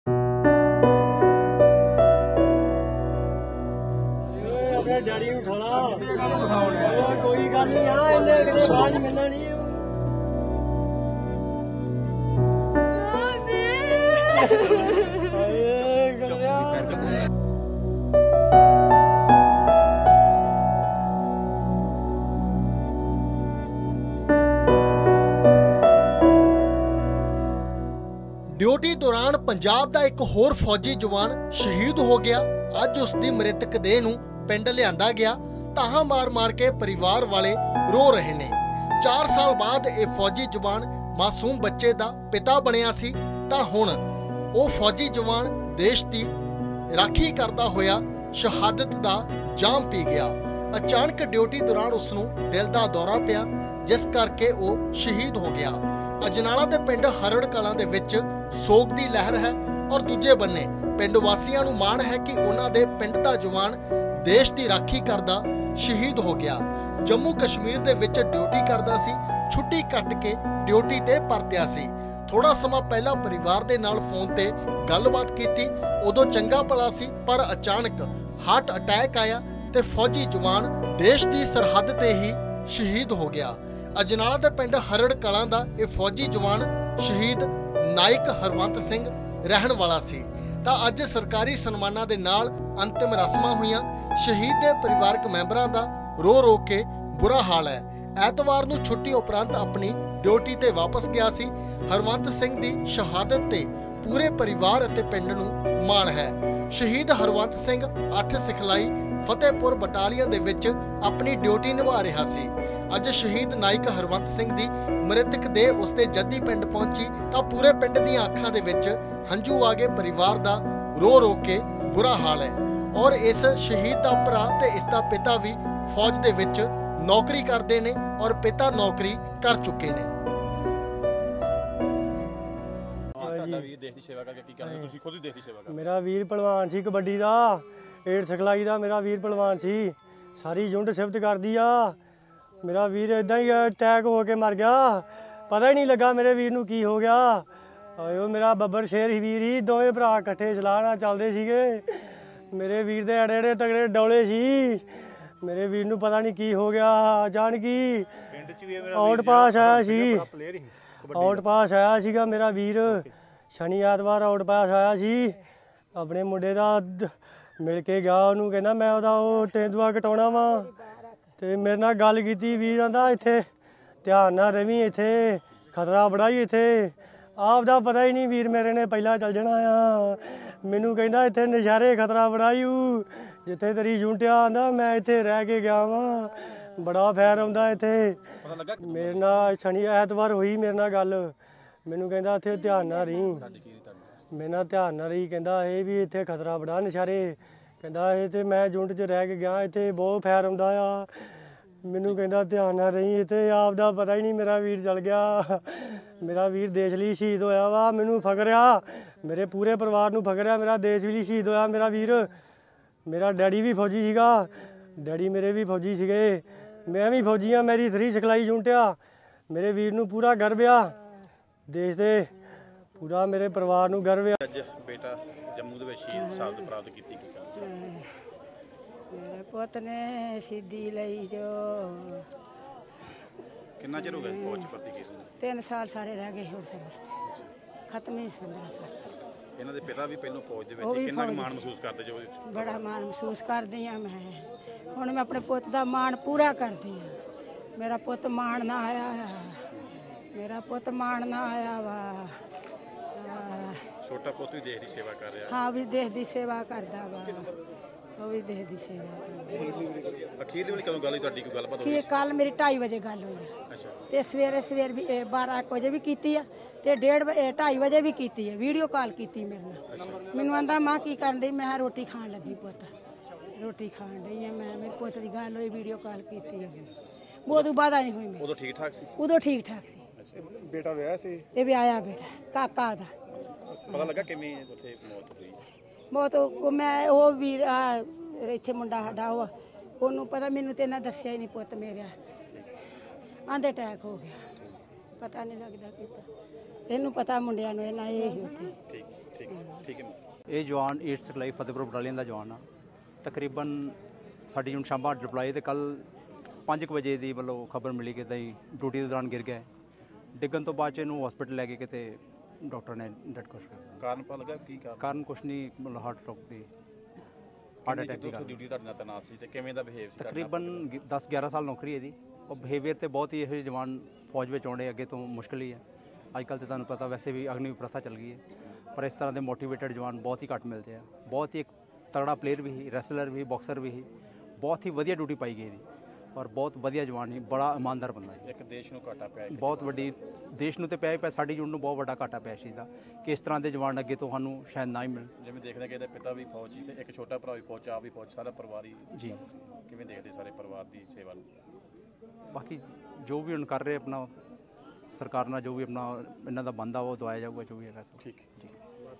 Amritsar News | ਦੇਸ਼ ਦੀ ਸਰਹੱਦ 'ਤੇ ਸ਼ਹੀਦ ਹੋਇਆ ਪੰਜਾਬ ਦਾ ਫੌਜੀ ਜਵਾਨ
ਜੰਮੂ ਕਸ਼ਮੀਰ ਦੇ ਸਾਂਭਾ ਸੈਕਟਰ ਵਿੱਚ ਤੈਨਾਤ ਅਜਨਾਲਾ ਦੇ ਪਿੰਡ ਹਰੜ ਕਲਾਂ ਦੇ ਰਹਿਣ ਵਾਲੇ ਫੌਜੀ ਜਵਾਨ ਹਰਵੰਤ ਸਿੰਘ ਦੀ ਡਿਊਟੀ ਦੌਰਾਨ ਅਚਾਨਕ ਦਿਲ ਦਾ ਦੌਰਾ ਪੈਣ ਕਰਕੇ ਸ਼ਹੀਦ ਹੋ ਗਿਆ।